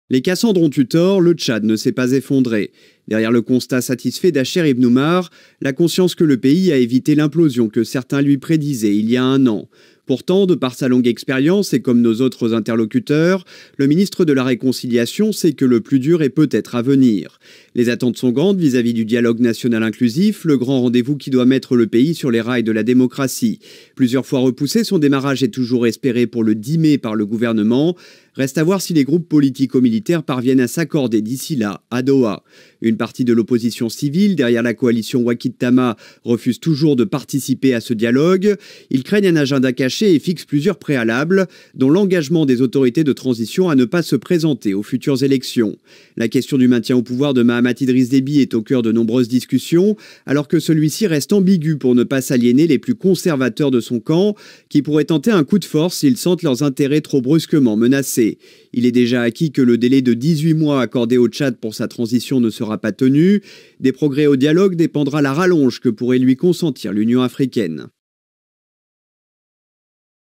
Tchad - Enquête sur la grande énigme [4/4] - le défi de la paix et de la liberté • RFI
Un an après la mort d’Idriss Déby Itno, le Conseil militaire de transition créé dans l’urgence est toujours en place. Le pays attend un dialogue national inclusif qui doit permettre de se projeter sur l’avenir. La population s’interroge cependant sur les projets de Mahamat Idriss Déby, qui dirige le CMT, et sur la volonté des militaires de rendre le pouvoir aux civils. Dans ce quatrième et dernier volet de notre enquête, nous revenons sur les inquiétudes et les espoirs des Tchadiens.